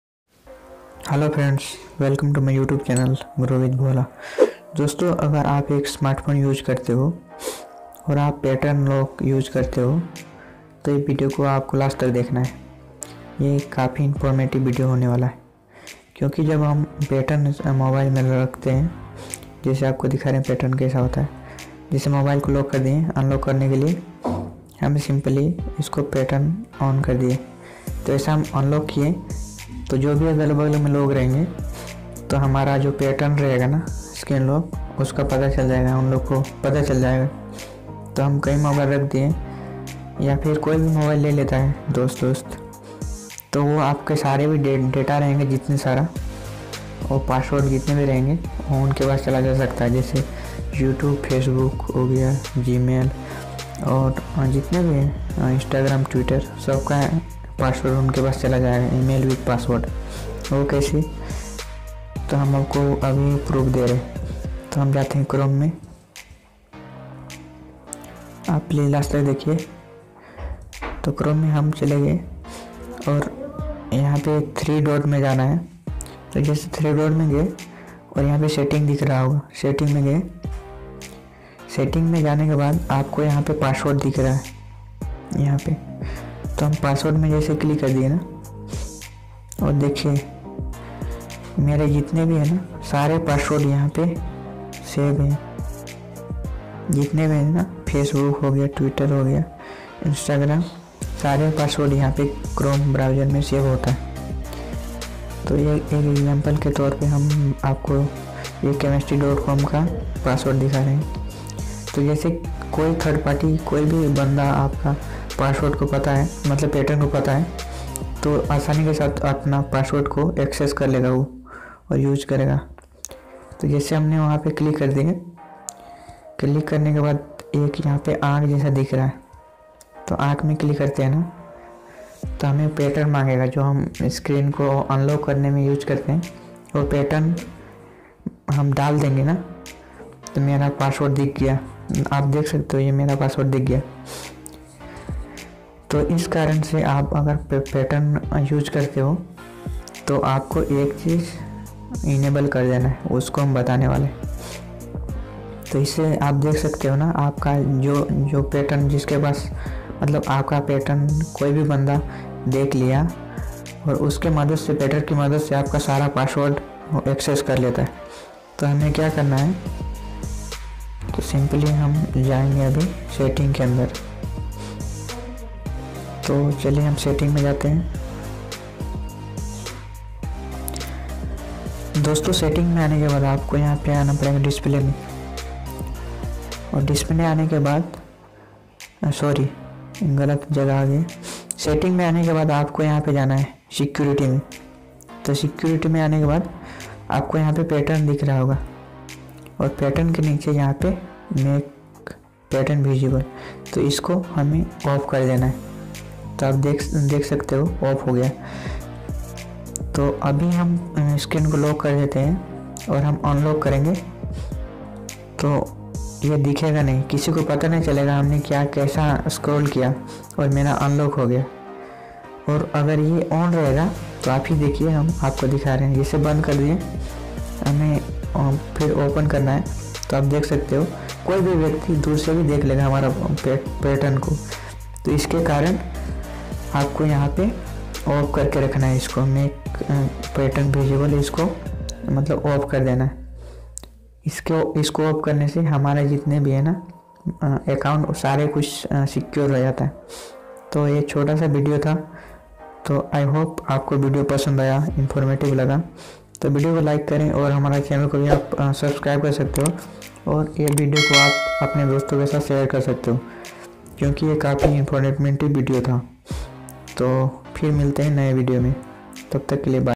Pattern Lock | Hidden Setting In Pattern Lock | How To Secure Personal Data On Mobile Phone
Pattern Lock | Hidden Setting In Pattern Lock | How To secure Personal data | Mobile Tips And Tricks |

Hey Friends welcome to my YouTube
channel "Grow With Bhola" I hope you all are   doing good

       Dosto is video me maine aapko ye btaya hai ki aap apne pattern lock ko kaise hide kar sakte hai  |  psttern lock agar kisi ko pta chal gaya to apke sare account hack ho sakta hai jaise Facebook account Gmail account Instagram account Twitter account YouTube account sare account ka password kise ko pta chal jayega isliye aapko hamesa mobile phone ke pattern ko unvisible mode ko on kare rakhna hai

1) Pattern unvisible
2) Pattern lock
3) Patern lock unvisible
4) pattern lock visible
5) pattern lock ko unvisible karne ke fayde
6) Secure your data